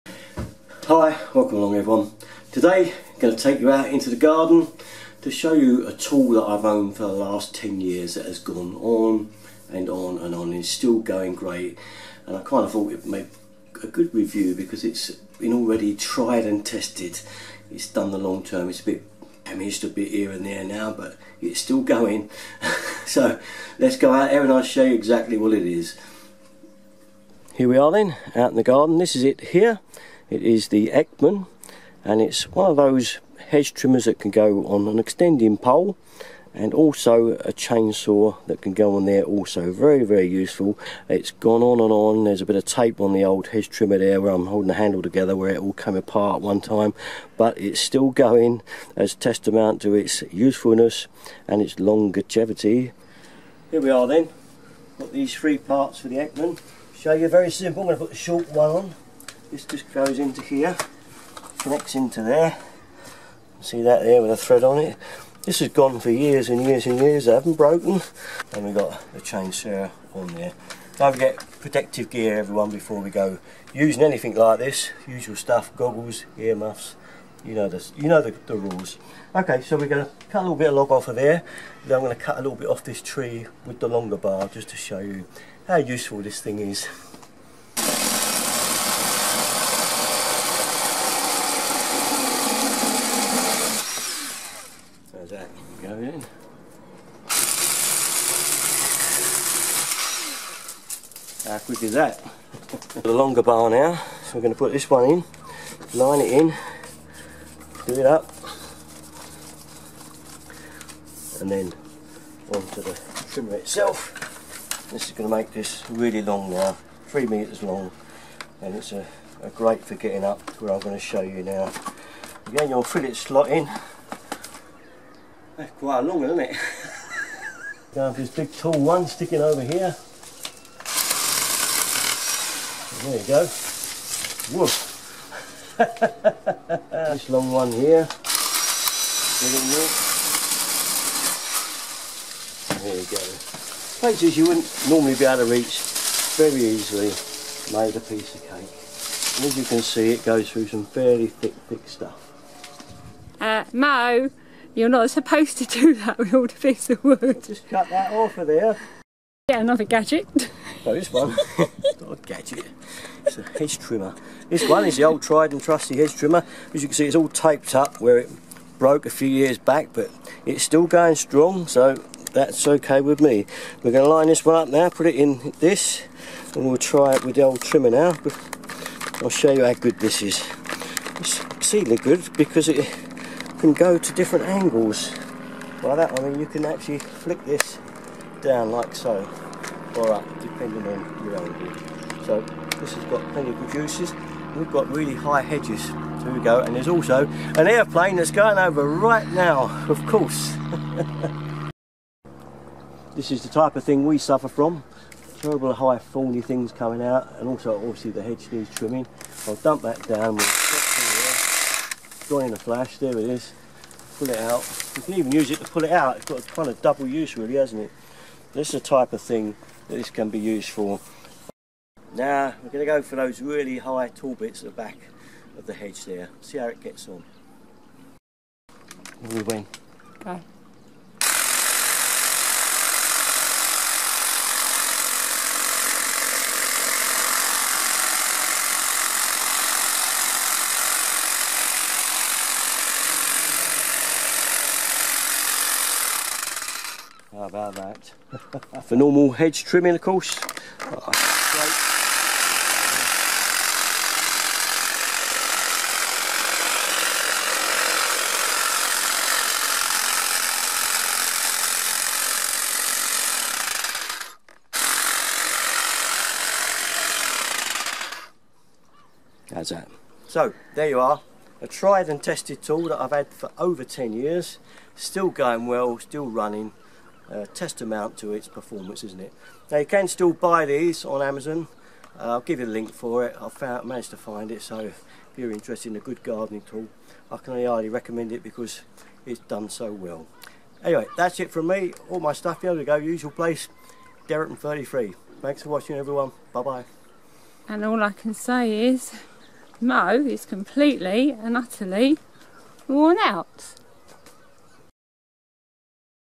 Review of my 10 year old Eckman extending hedge and chain saw cutter
Some tools are worth a mention because they have stood the test of time. This is one of them.